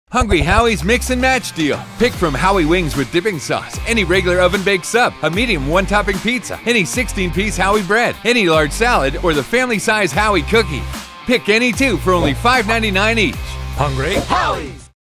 Hungry Howie's Pizza| Mix & Match| :15 Sec
Get a pizza deal any way you like it with our Mix & Match offer! Choose between pizza, subs, bread, wings or salad for only $5.99 each!